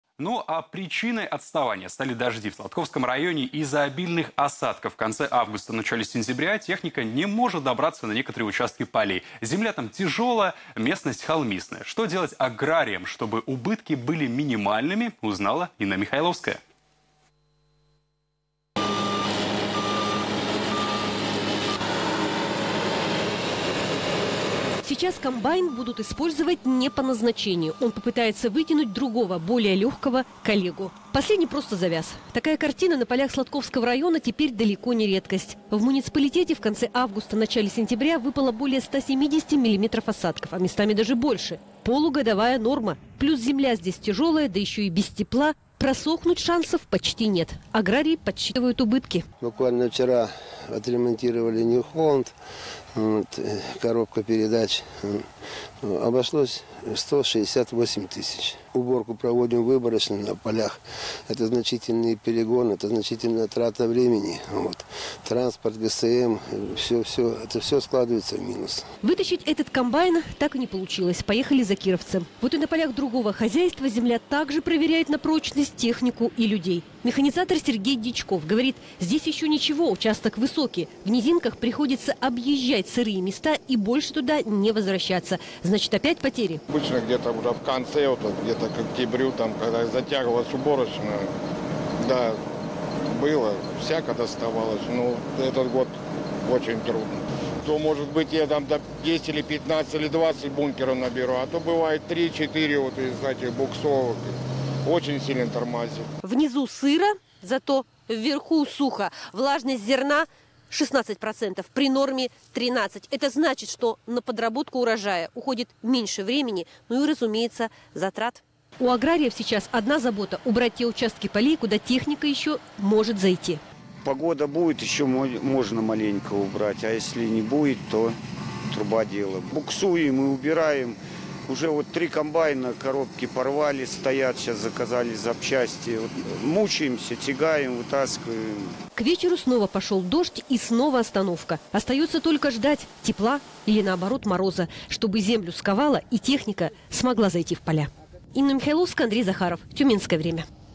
Битва за урожай
В Сладковском районе из-за обильных осадков в конце августа - начале сентября, техника не может добраться на некоторые участки полей. Земля там тяжелая - местность холмистая. Что делать аграриям чтобы убытки были минимальными?